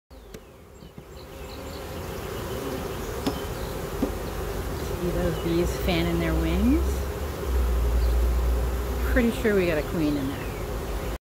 Swarm 2 Fanning
The bees are letting the rest of their swarm know where the queen is.